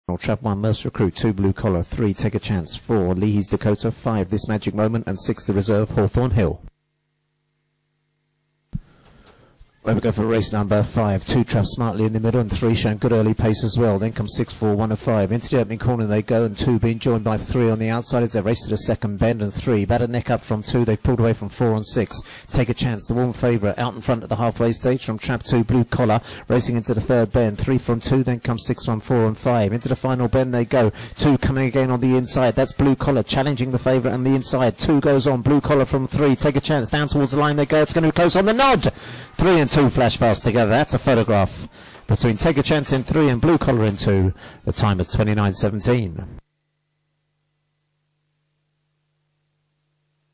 IGB - GMHD Insurances A6 Stakes Final 08/03/2019 Race 5 - Tralee
Race: GMHD Insurances A6 Stakes Final
Track Name: Tralee
Date: 08/03/2019
Hour: 20:42

Results

1st    3     TAKE A CHANCE       4/6f         29.07
2nd    2     BLUE COLLAR         5/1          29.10
3rd    5     THISMAGICMOMENT     5/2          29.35
4th    1     MURPHS RECRUIT      5/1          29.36
5th    6     HAWTHORN HILL       5/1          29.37
6th    4     LEAHYS DAKOTA       4/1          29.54

|TRL_2019_03_08_05